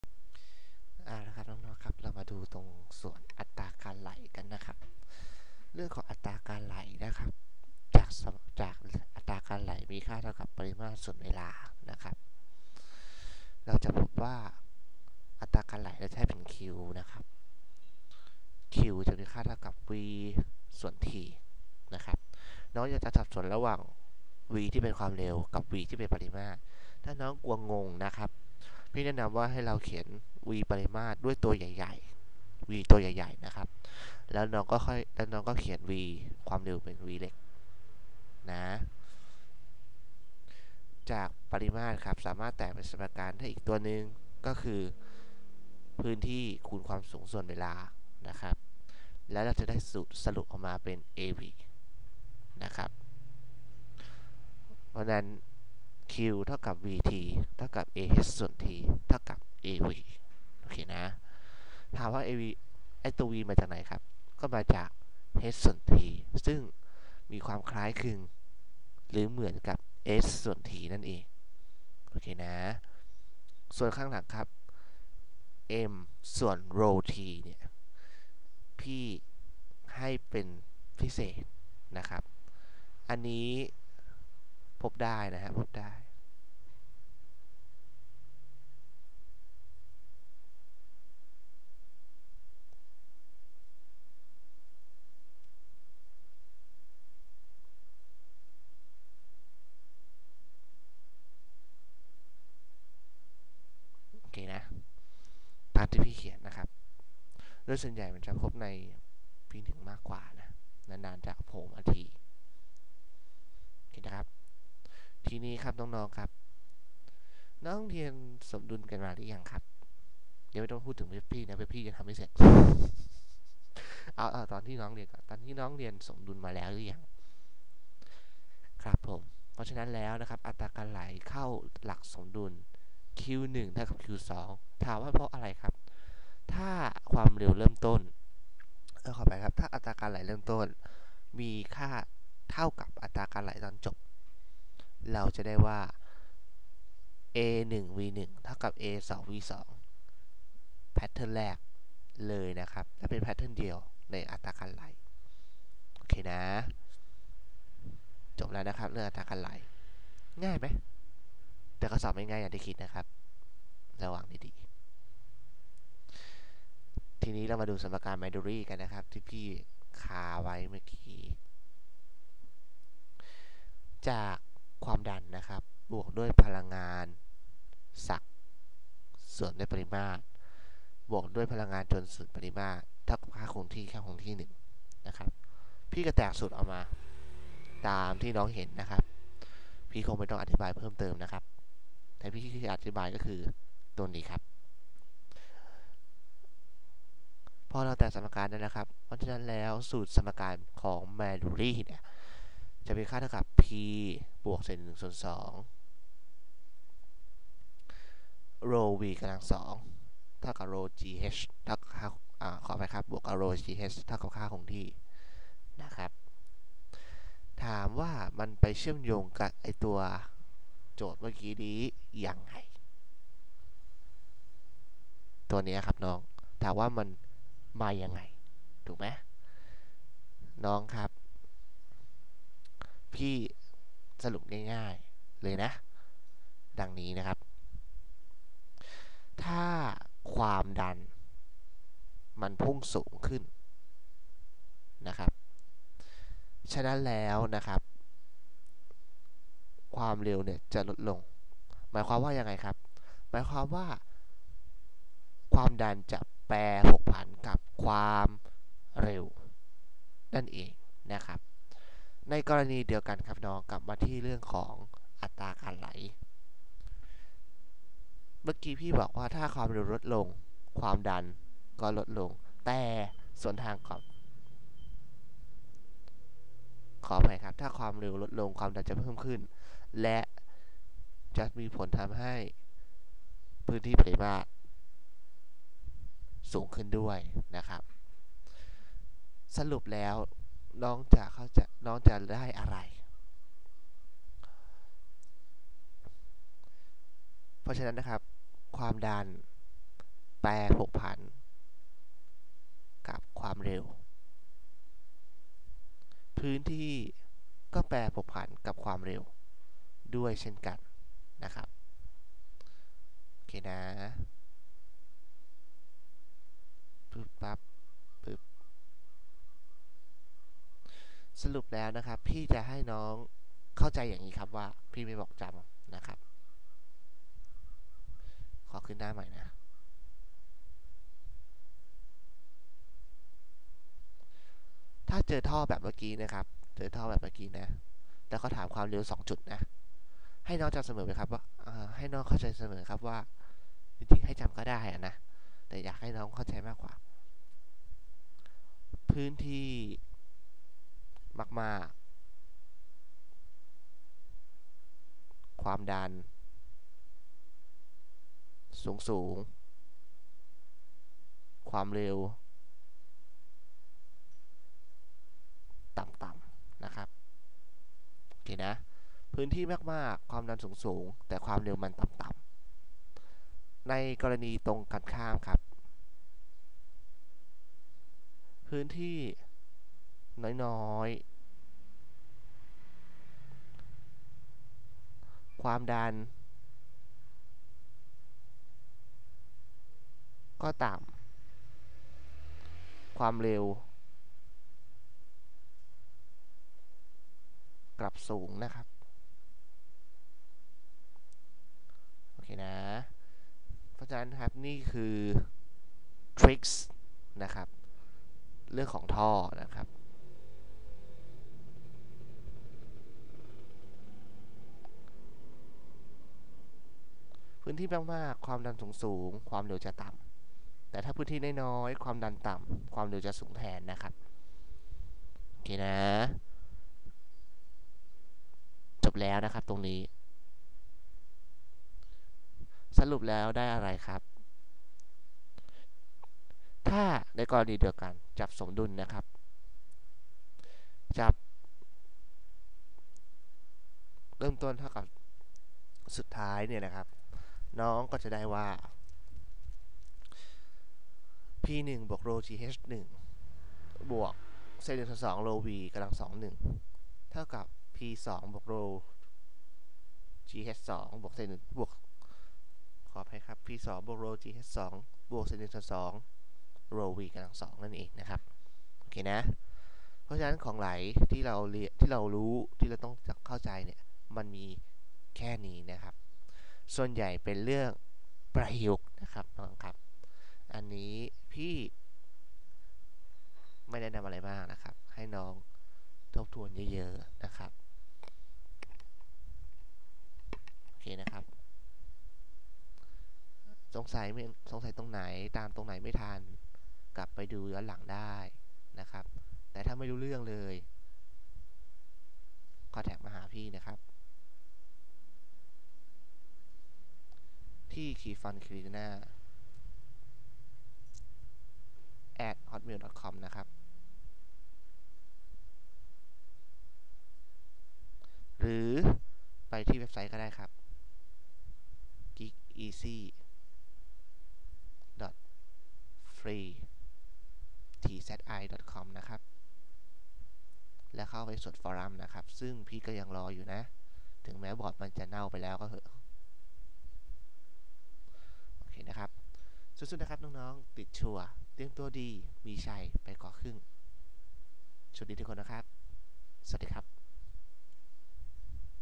สรุปฟิสิกส์ : ของไหล ตอนที่ 2
มาแล้วครับ พี่กิ๊กส่งตัวของไหลมาแล้วนะครับ

เรื่อง ๆ สั้นแต่มีการประยุกต์มาก ๆ เลยนะครับ

ต้องหาโจทยืทำมาก ๆ นะครับ จะได้รู้แนว ^_^

สู้ ๆ นะครับน้อง ๆ ติดชัวร์
เตรียมตัวดีมีชัยไปกว่าครึ่ง